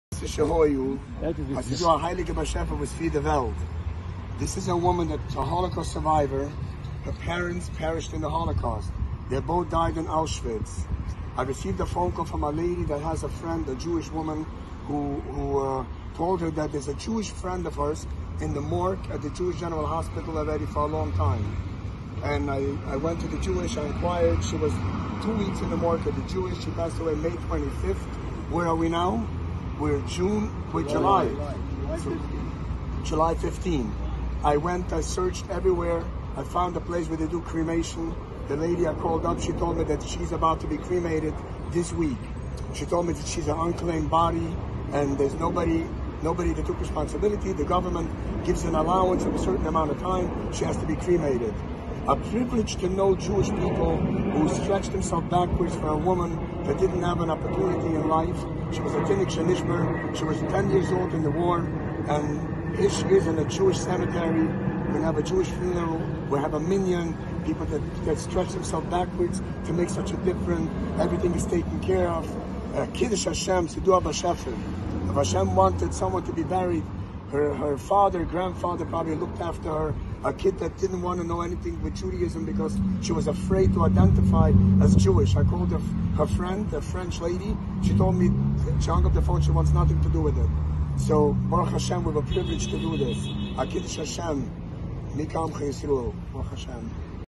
AUSCHWITZ SURVIVOR SAVED FROM CREMATION AND GIVEN JEWISH FUNERAL
MTC's L'Chaim Project provides a Minyan for Kaddish at the funeral of a women who was so traumatized by the Holocaust that she hid her Jewish identity, even changing her name.

As an "unclaimed body", she was almost cremated. Today, thank G-d, she was laid to rest as a Jewish women, with a Minyan and Kaddish.

BIG Yasher Koach to Aaron Schmelczer for making this happen!!
The L'Chaim Project is a pool of volunteers who guarantee a Minyan at any funeral that might not have one otherwise.